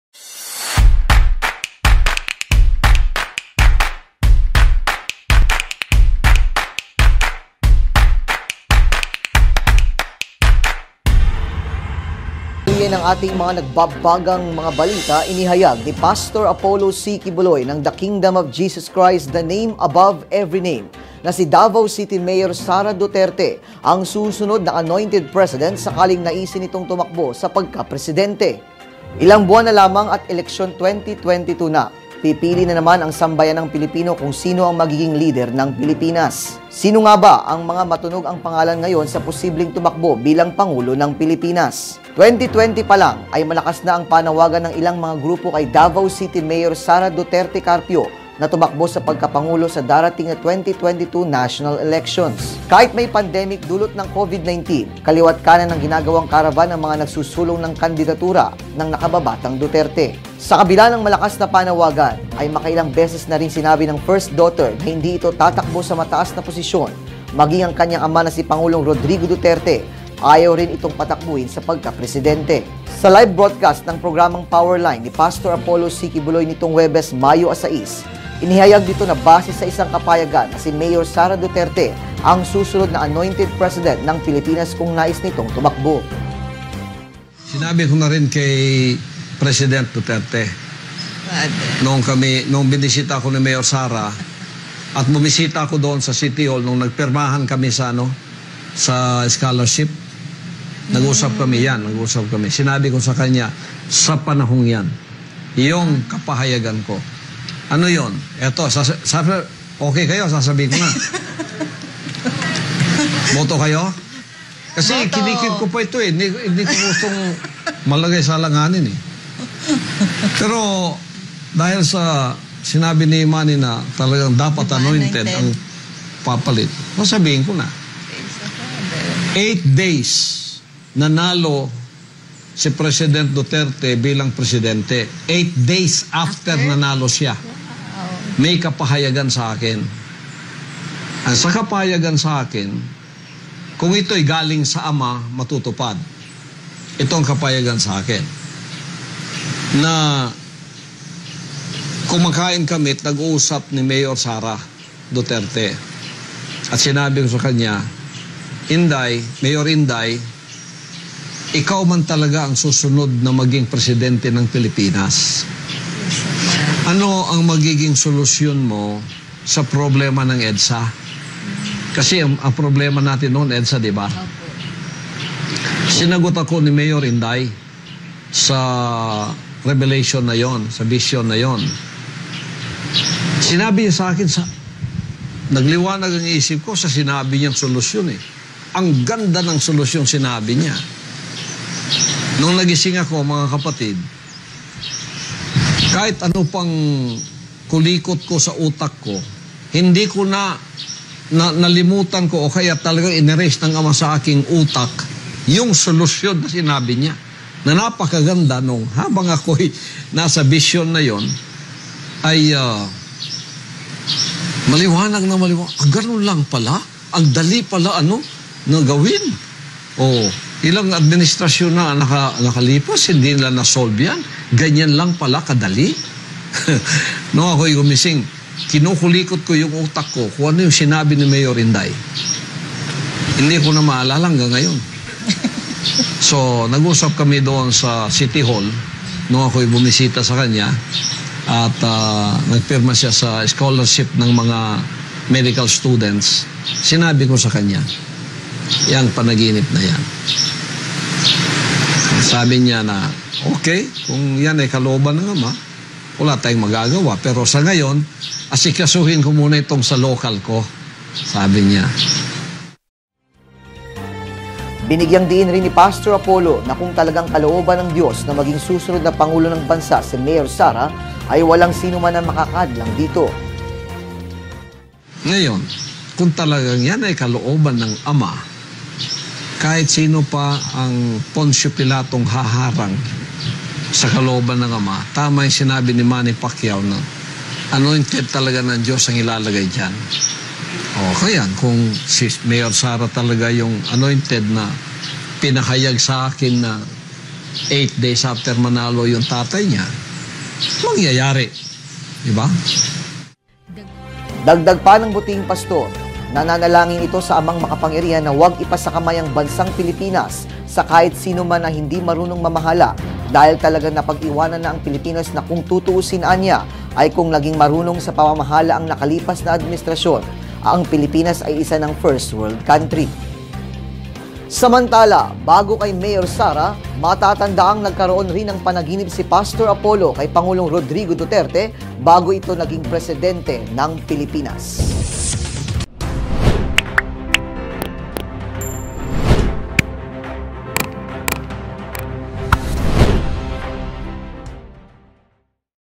Mayor Sara Duterte susunod na Annointed President , ayon kay Pastor Apollo C. Quiboloy
Inihayag ni Pastor Apollo C. Quiboloy ng the Kingdom of Jesus Christ The Name Above Every Name na si Davao City Mayor Sara Duterte ang susunod na Annointed President sakaling naisin nitong tumakbo sa pagka presidente.

Abangan pa ang maiinit na balita dito lang sa